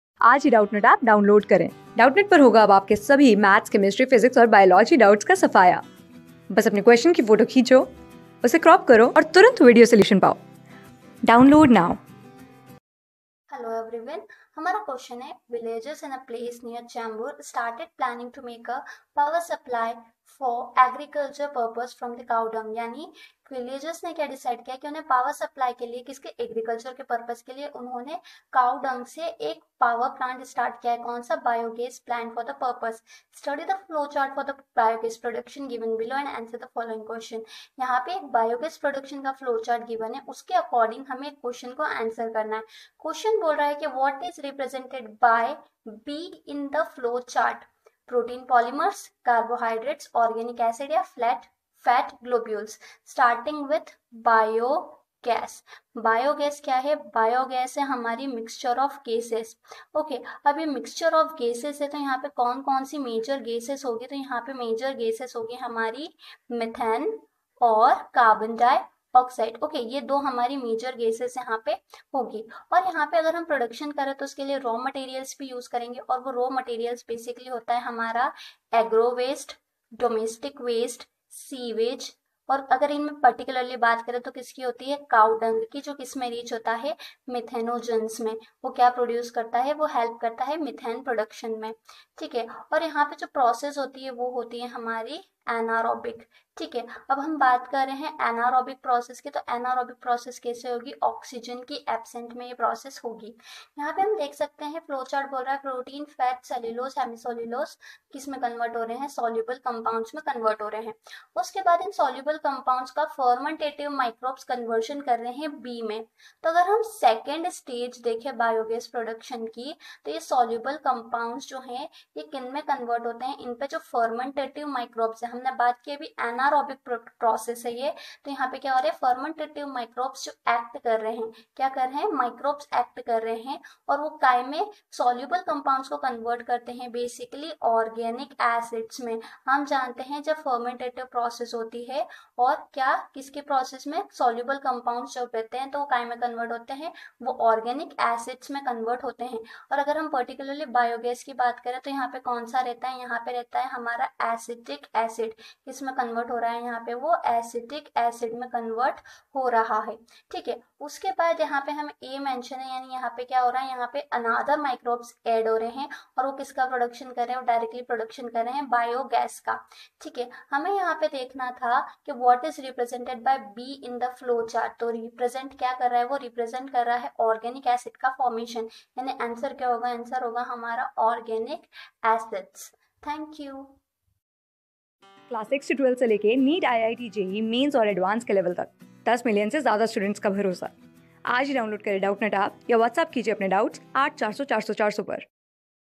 Villagers in a place near Chambur started planning to make power supply for agricultural purpos...
Villagers in a place near Chambur started planning to make power supply for agricultural purpose from cow dung. They have started a biogas plant for the purpose. Study the flow chart for biogas production given below and answer the following questions.What is represented by \'B\' in the flow chart?
Class: 12
Subject: BIOLOGY
Chapter: MICROBES IN HUMAN WELFARE
Board:CBSE

👉 Have Any Query? Ask Us.